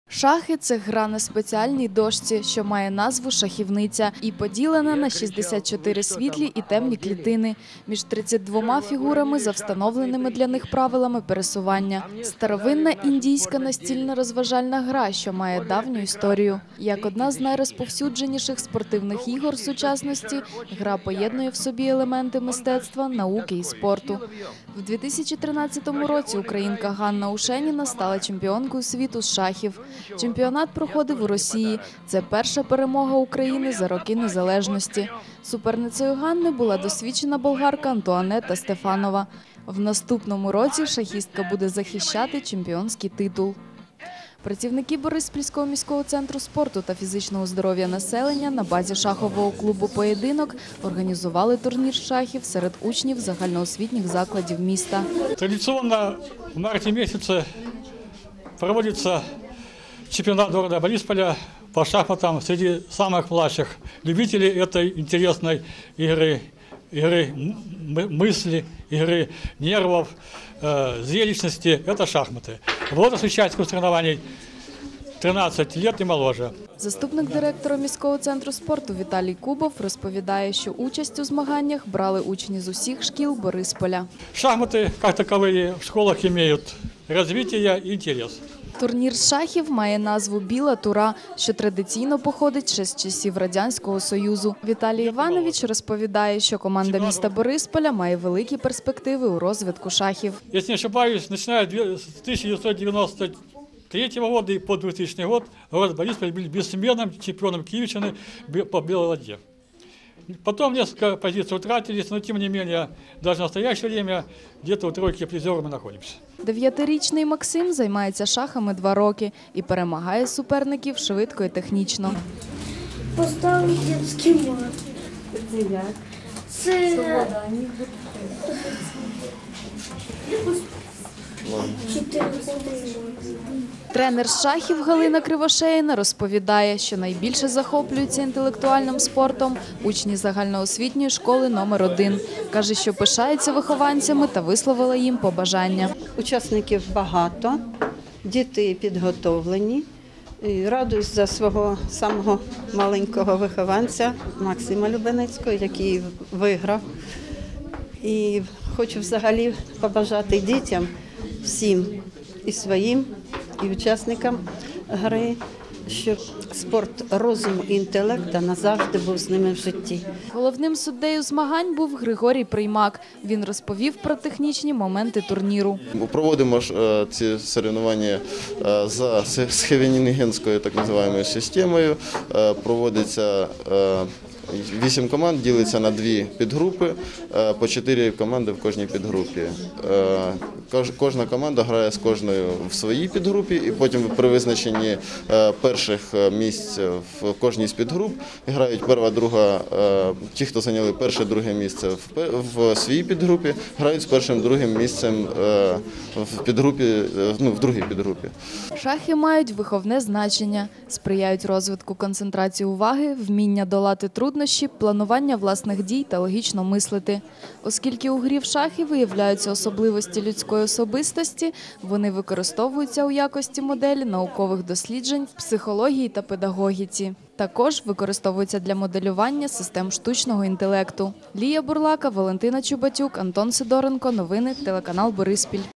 Інтелектуальний спорт
На базі шахового клубу "Поєдинок" у Борисполі організували турнір з шахів серед учнів загально освітніх шкіл міста